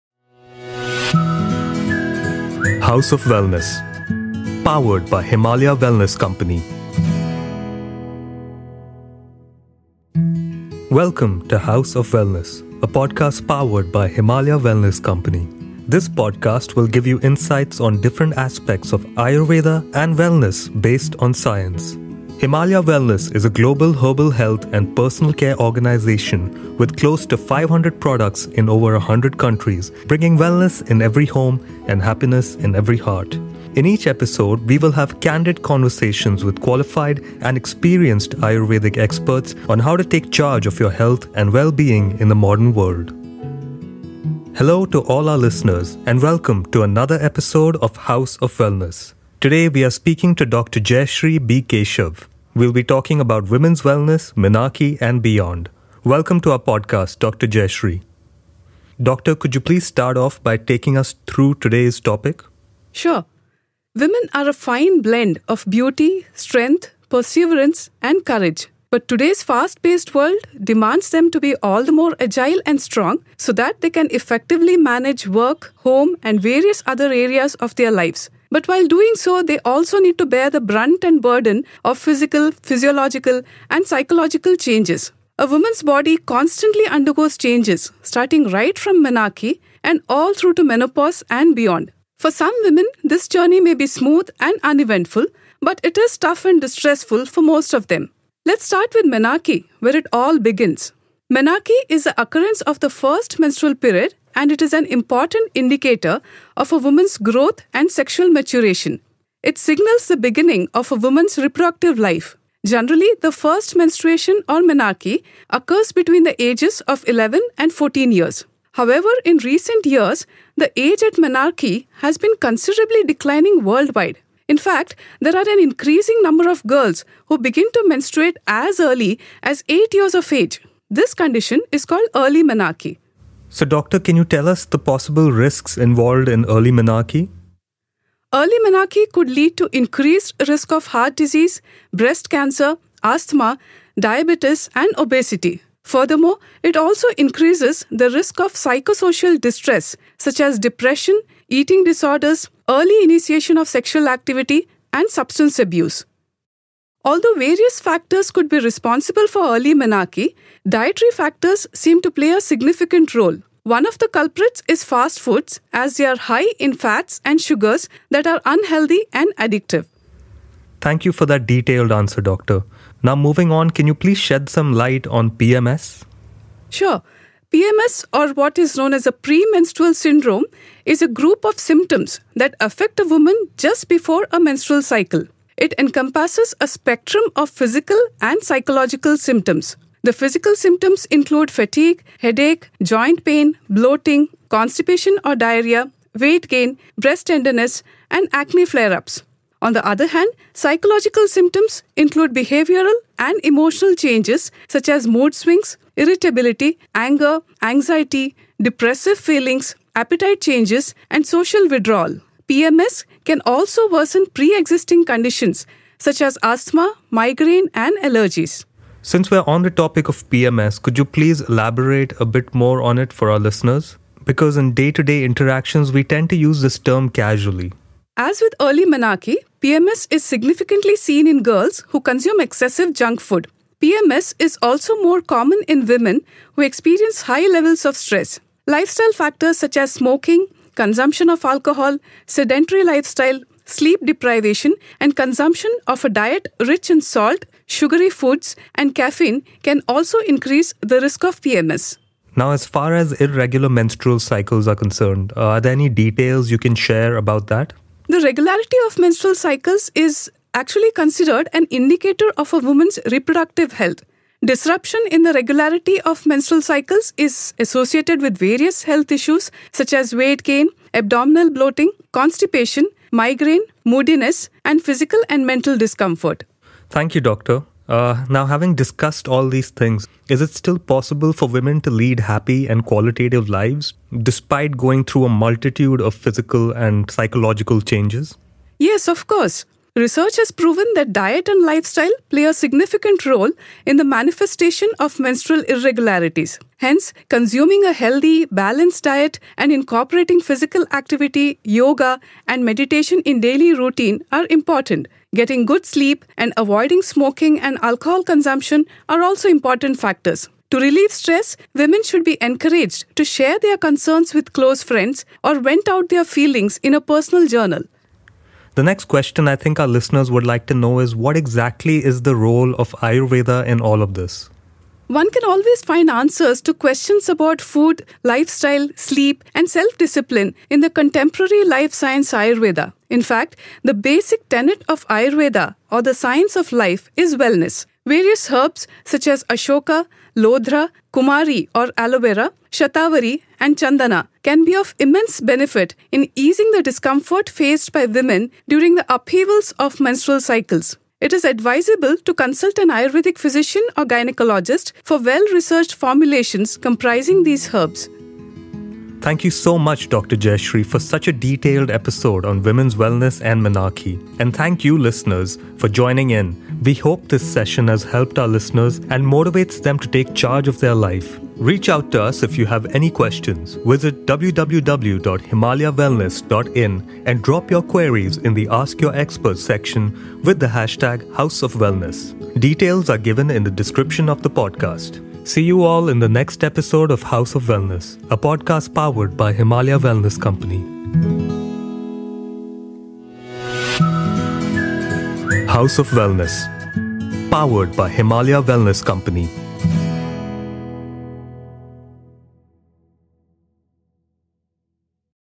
Early menarche and beyond; everything you need to know
Today's fast-paced world demands women to be even more agile and strong, so they can effectively manage work, home, and various other areas of their lives. While doing so, they also need to bear the brunt and burden of physical, physiological, and psychological changes—starting with menarche.

In the sixth episode of House of Wellness, Dr. Jayashree B Keshav shares insights on early menarche, menstrual irregularities, and the role of Ayurveda during the upheavals of menstrual cycles.